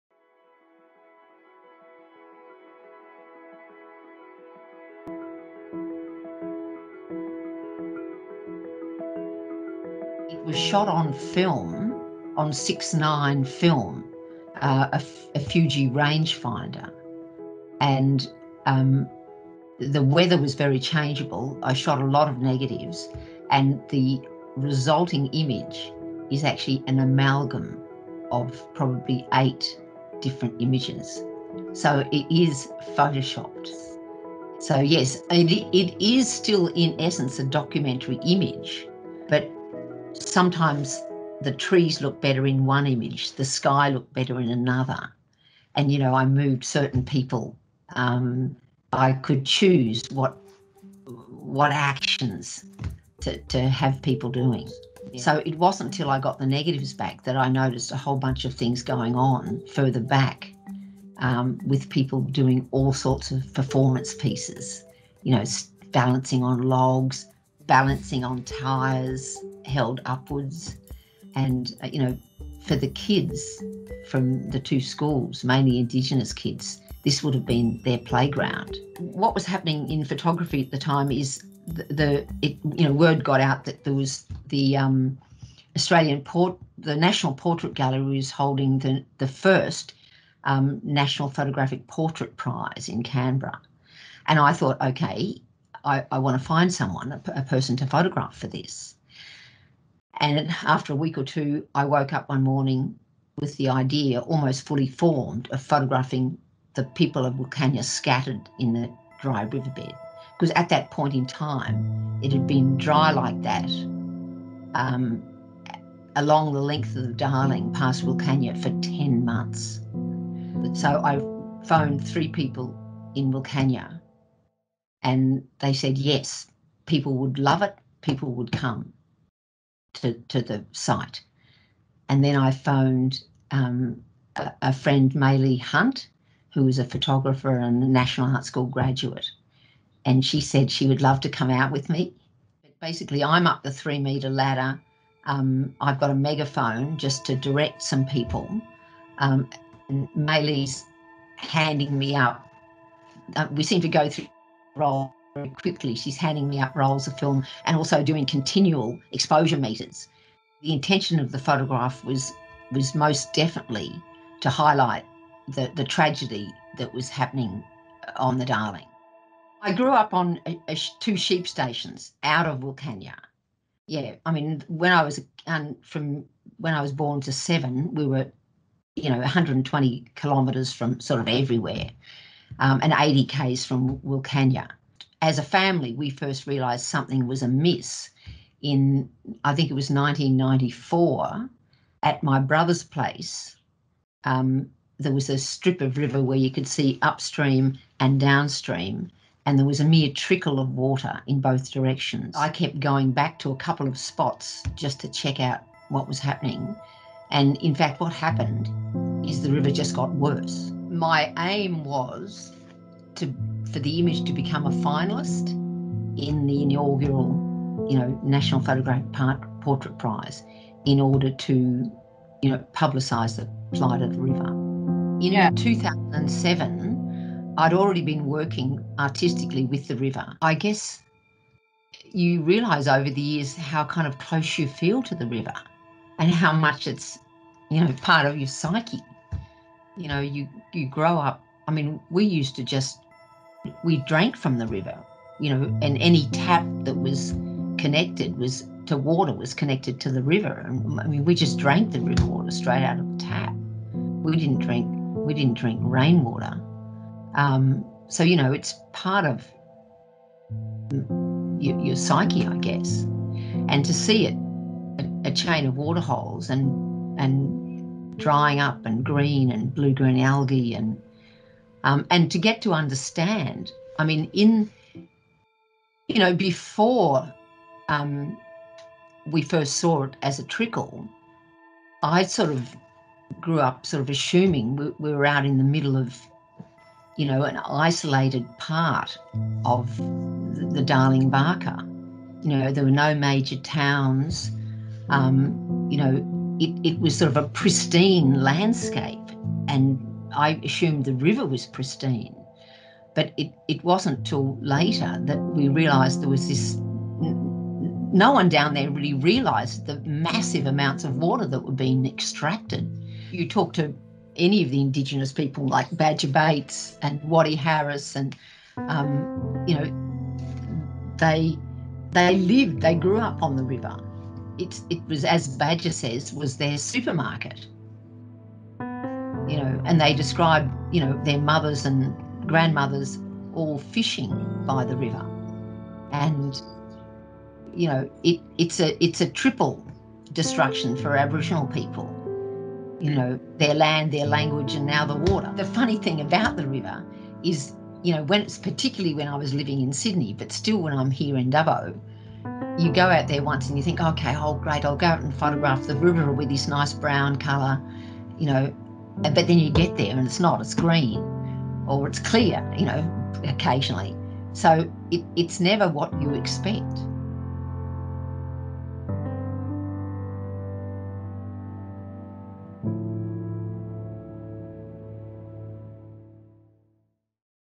The Object Account | Ruby Davies | Water as life: the town of Wilcannia on the Darling/Barka
Join artist Ruby Davies in this episode of The Object Account as she recounts the making of her photograph Water as life (2007), her collaboration with the community of Wilcannia, and the river as subject.